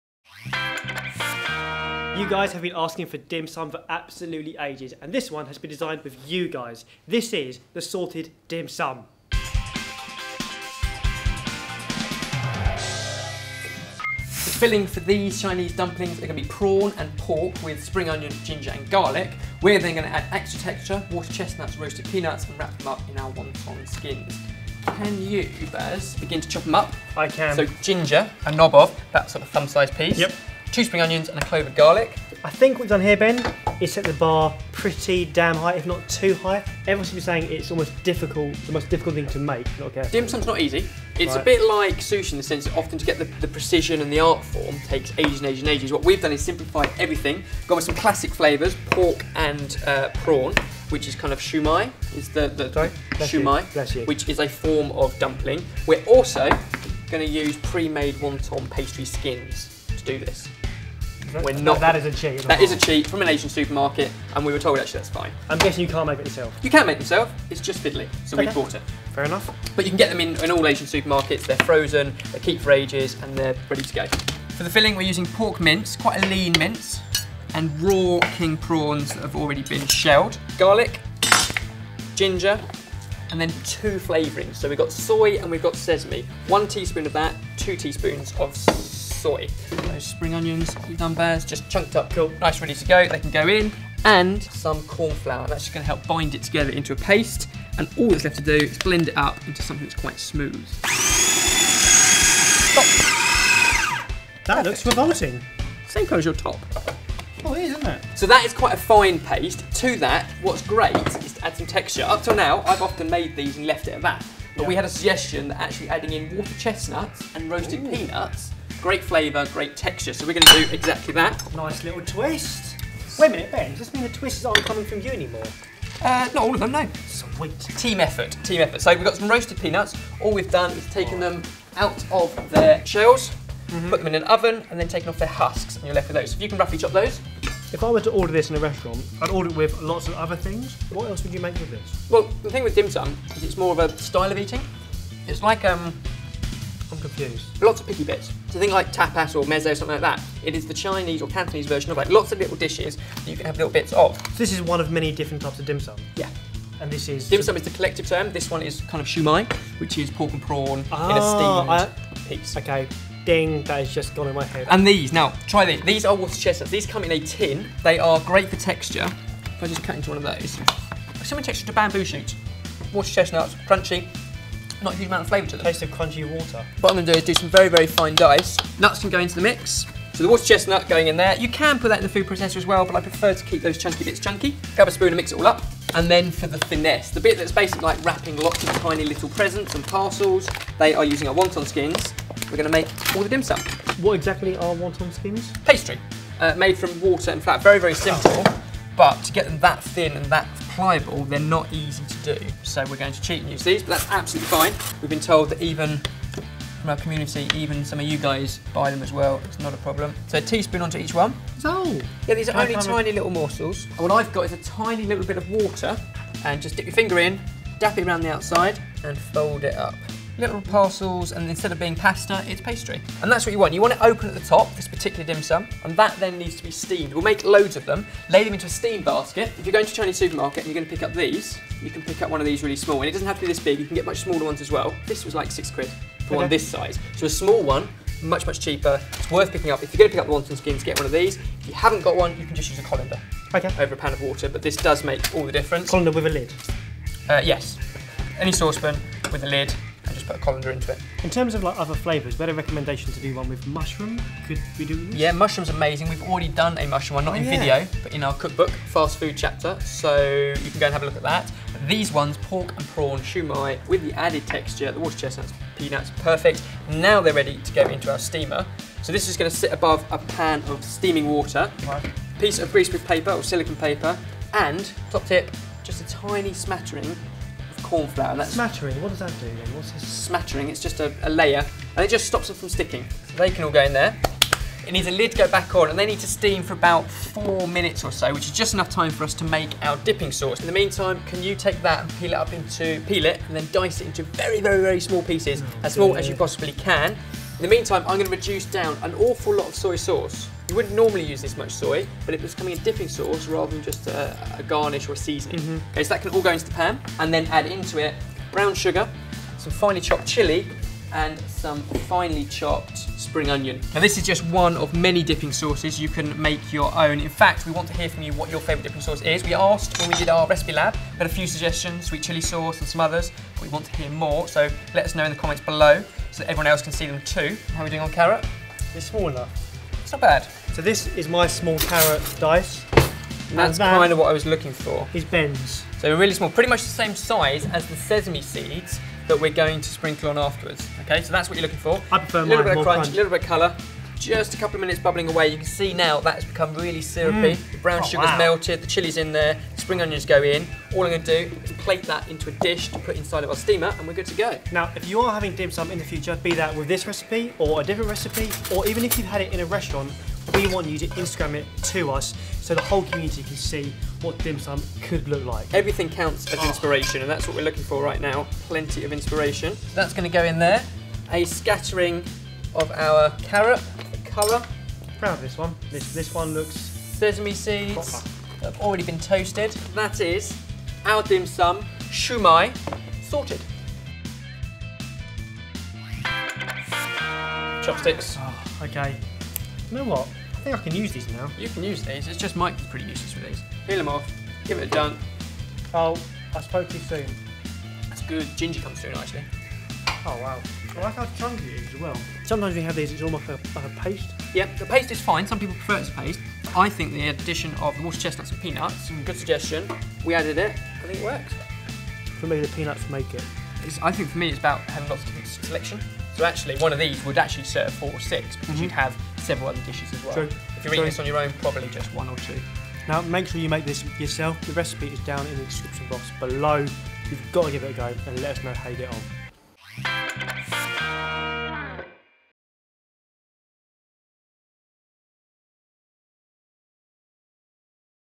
Chinese Dim Sum Recipe - SORTED
We've had so many requests for a Dim Sum recipe and you've given us loads of tips and tricks to make them as magical as possible... So here's our version!!

So what do you think? Could you improve on our version? Let us know down below!

If you want to get involved and give us your recipe suggestions, head over to our Google+ community